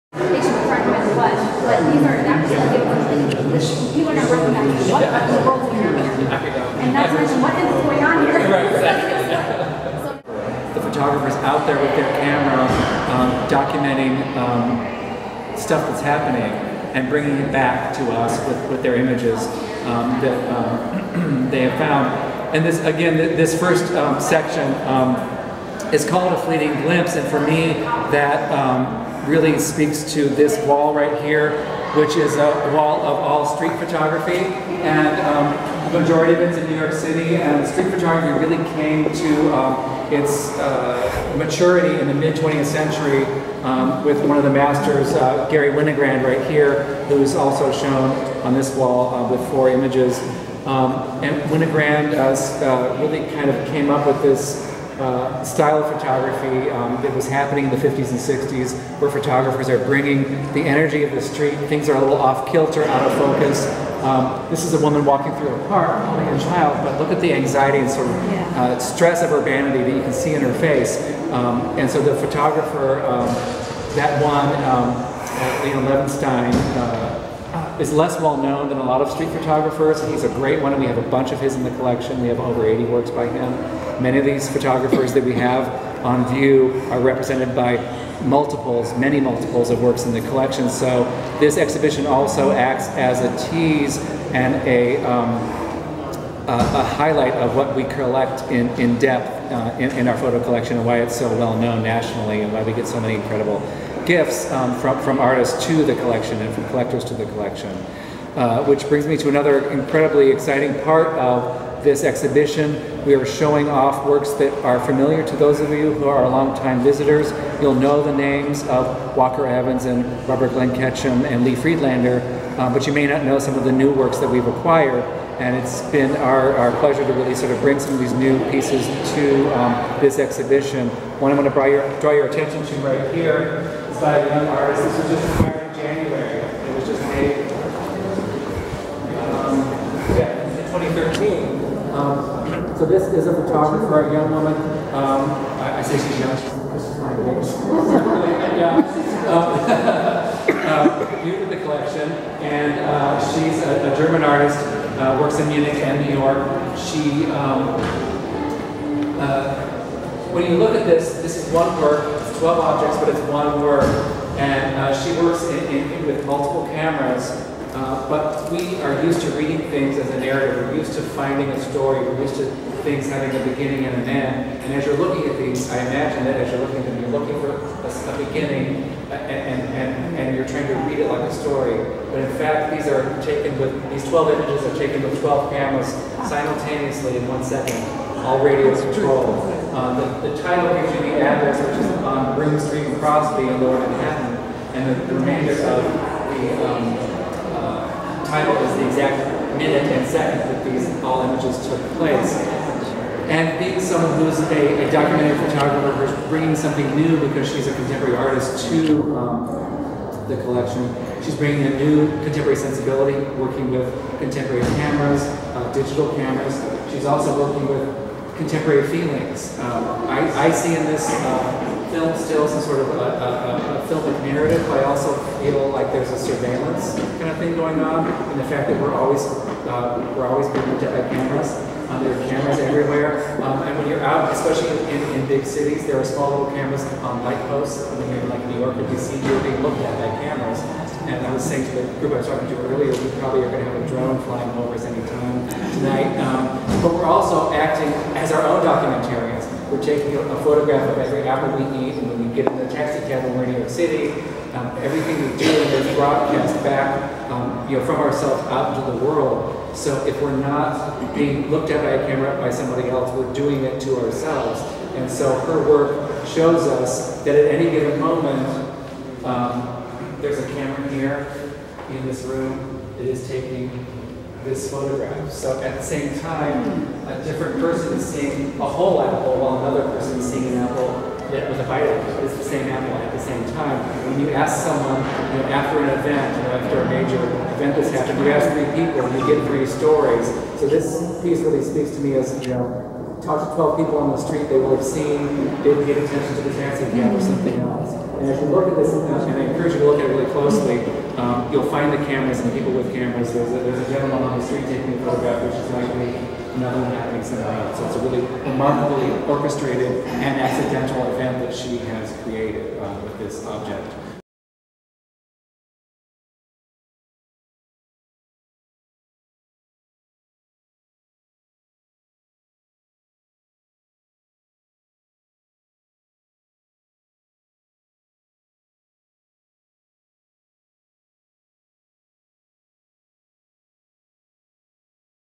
Proof at Akron Art Museum interview with Arnie Turnstall
Collections Manager Arnie Turnstall talks about the Akron Art Museum's newest exhibit, Proof, which highlights the museum's collection of documentary photography.